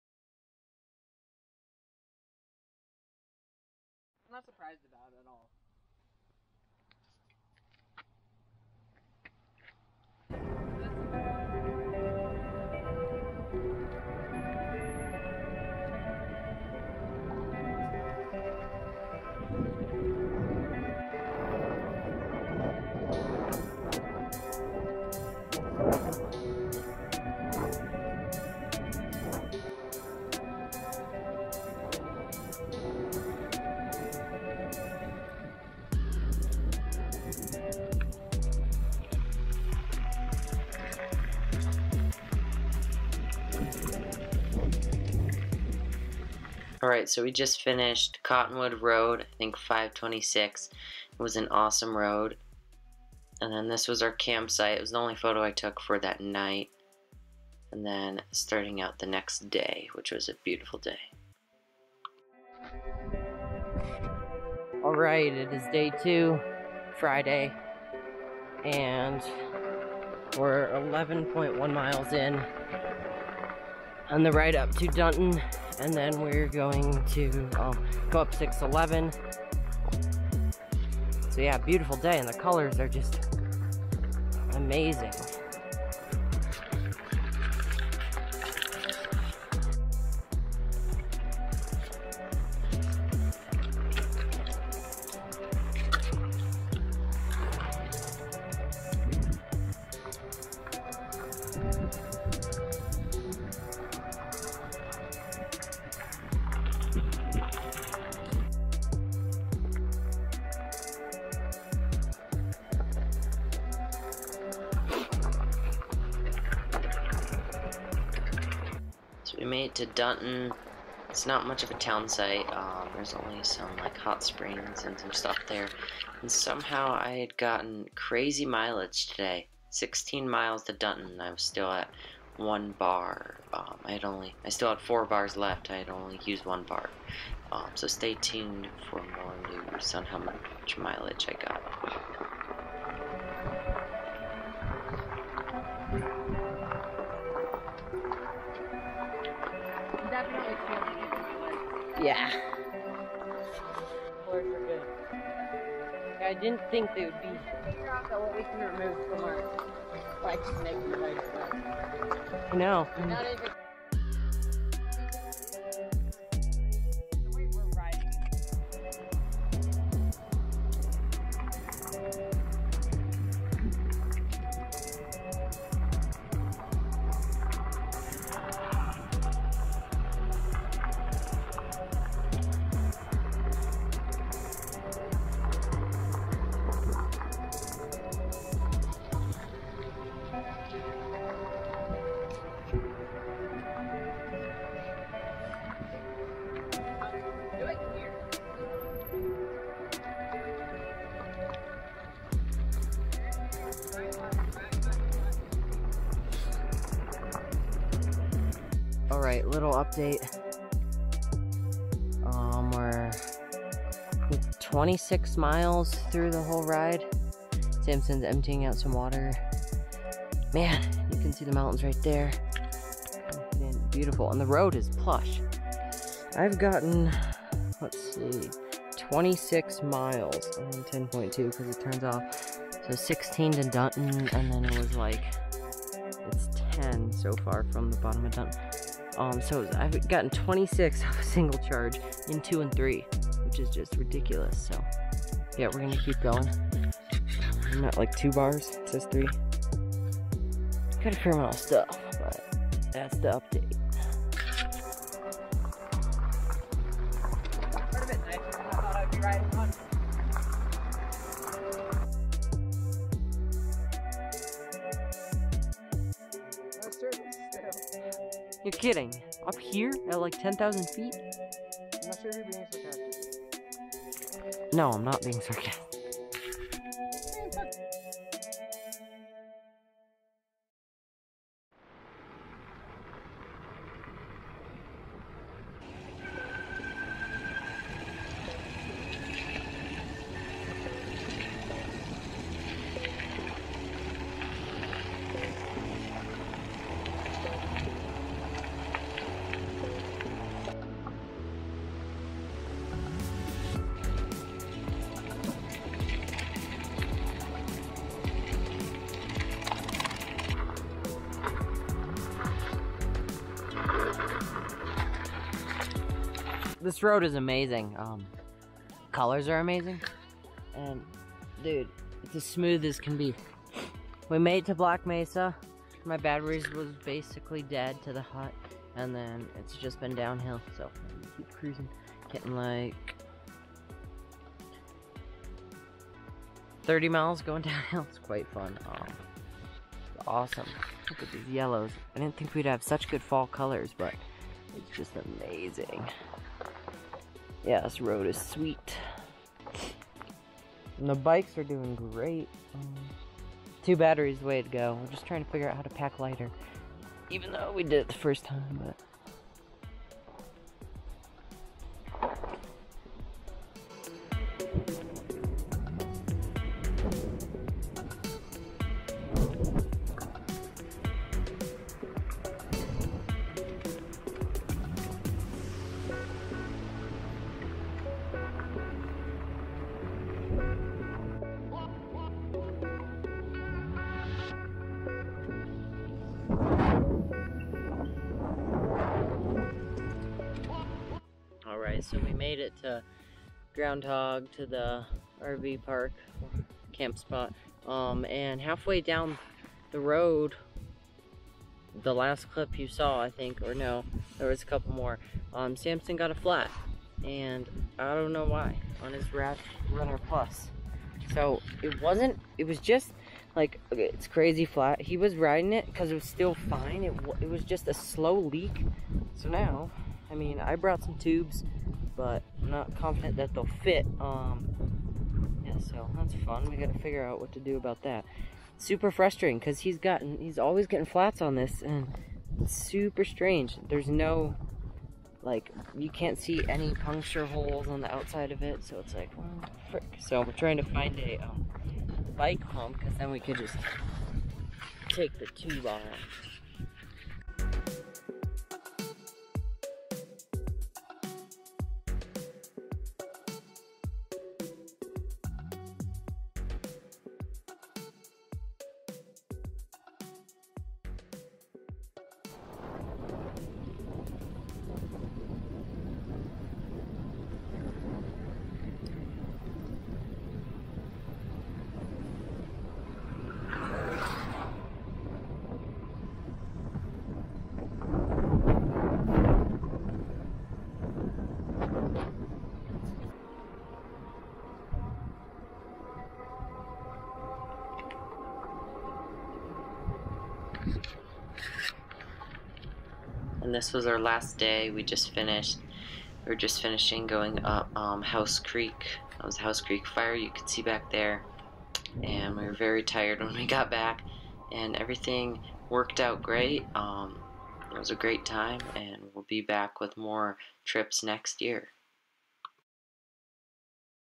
Ebike Packing 120 miles in 3 Days!
In this video you will see a 120 mile ebike packing trip me and a friend did. I had my Radwagon 4 and Samson with his radrunner plus, both bikes did great and it was so much fun! We will definitely be doing more trips next year!

and T2NATE